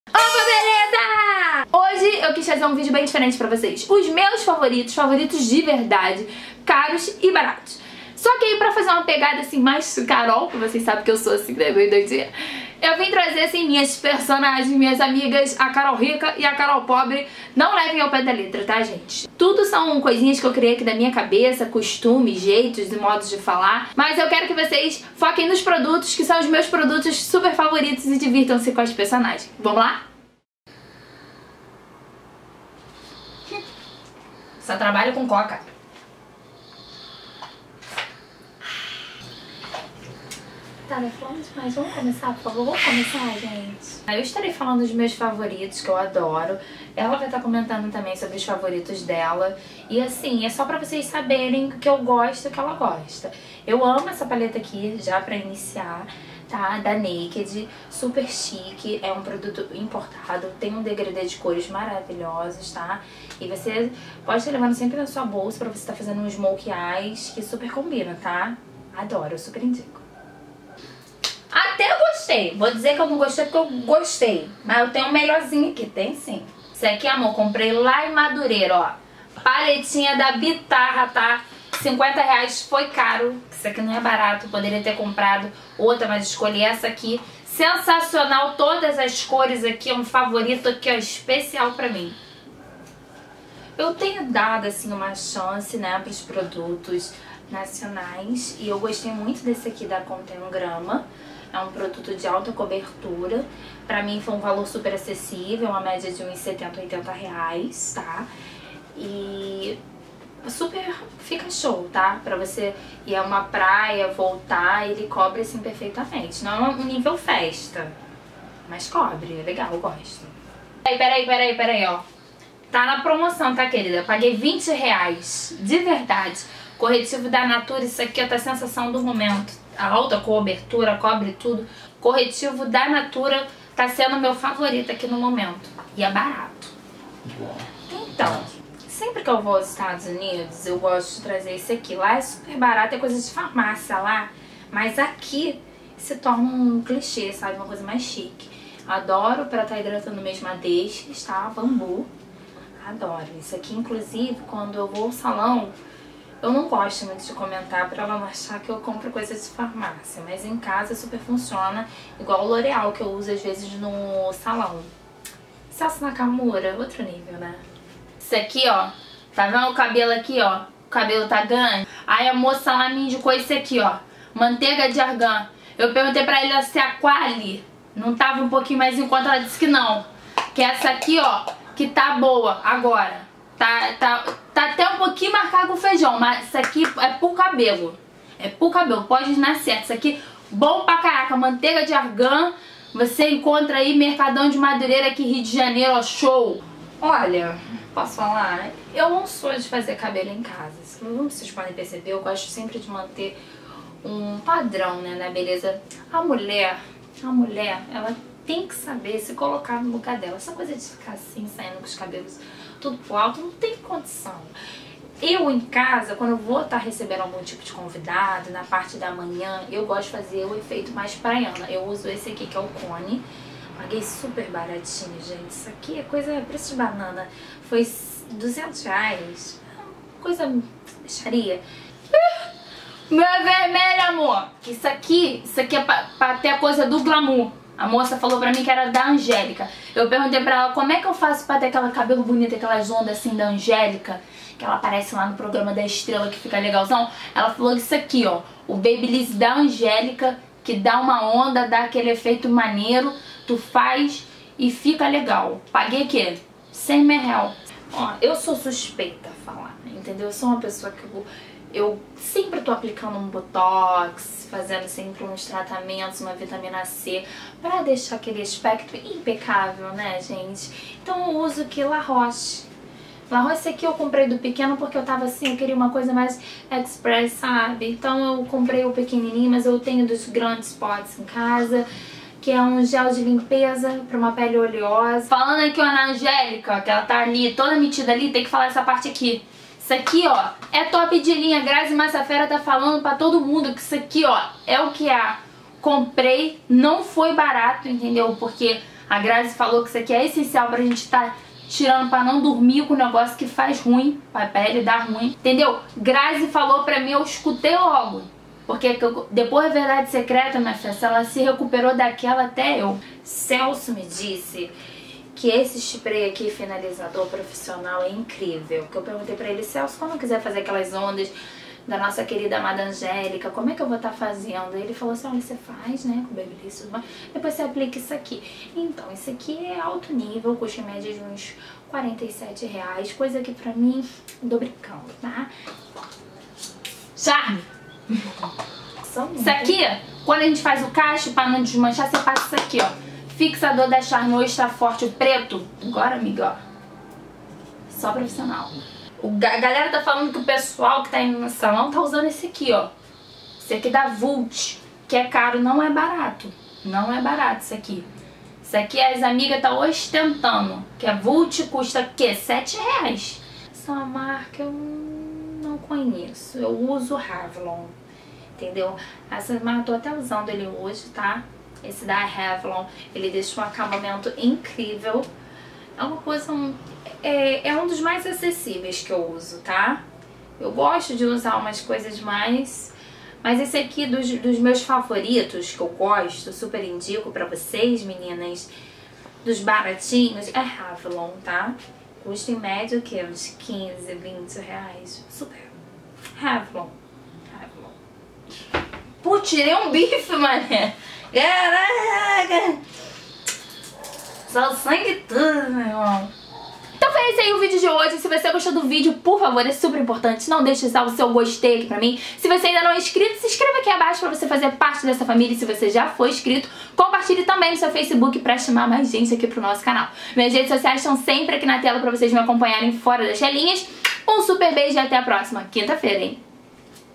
Favoritos BARATOS X CAROS (Carol Pobre x Carol Rica)
Hoje voltamos aos vídeos normais (TODAS SEGUNDAS E QUINTAS 20:00)

Hoje o vídeo ta super divertido e mostra 2 CAROLs! POBRE confrontando a RICA falando sobre produtos de beleza muito BONS E DE BOA QUALIDADE!